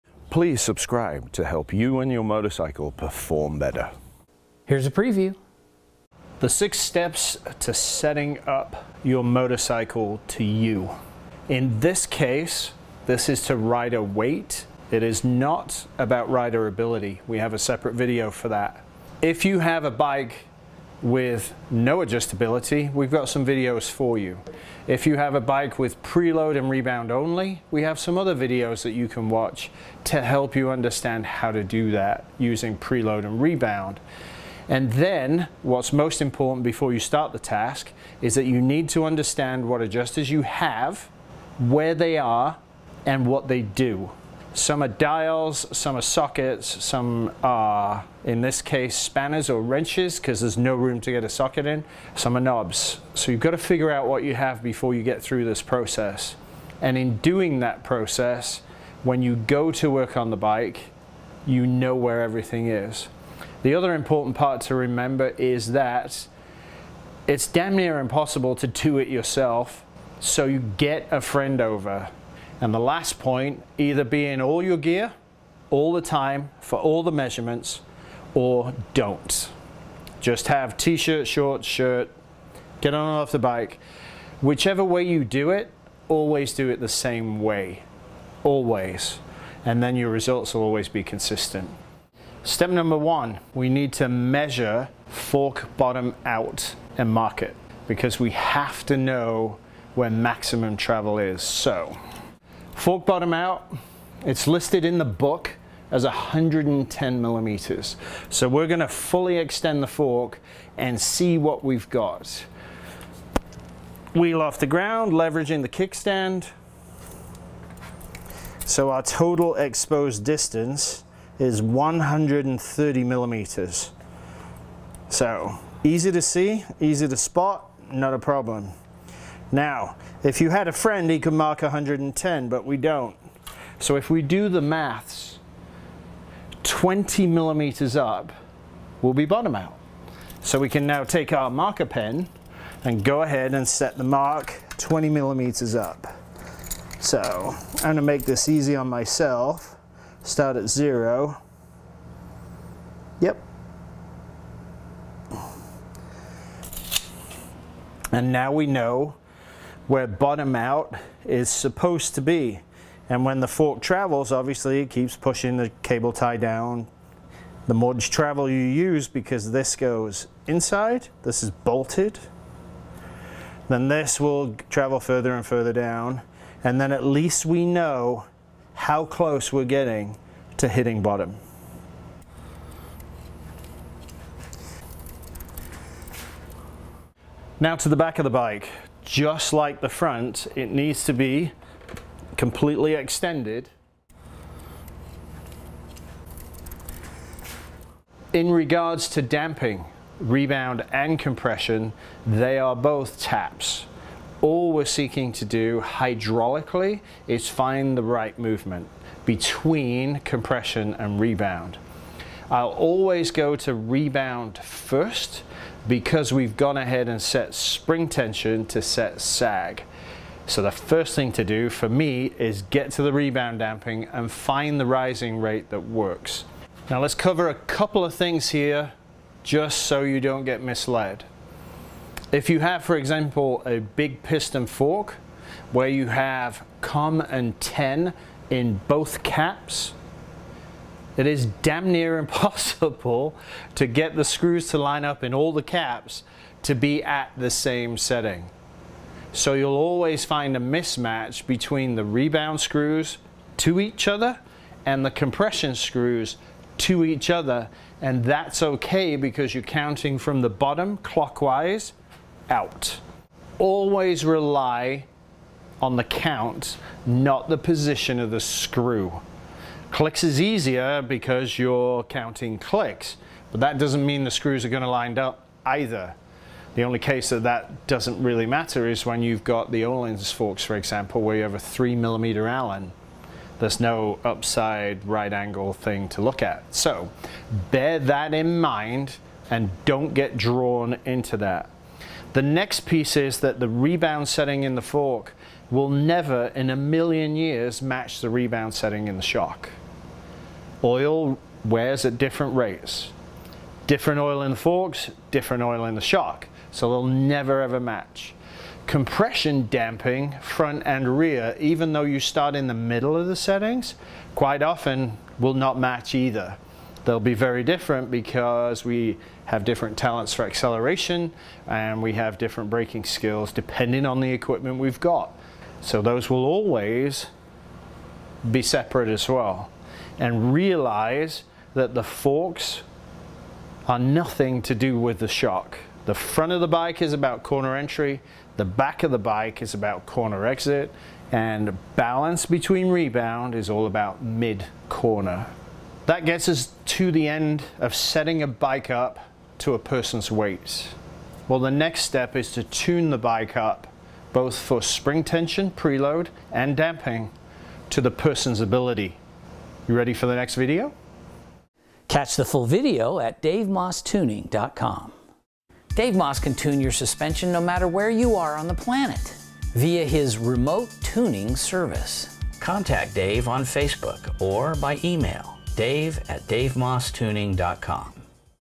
Set Up Your Motorcycle Suspension in 6 Easy Steps Intro
Your motorcycle came with adjustable suspension for a reason. In 6 easy steps you can have your suspension working perfectly and giving you the best ride of your life.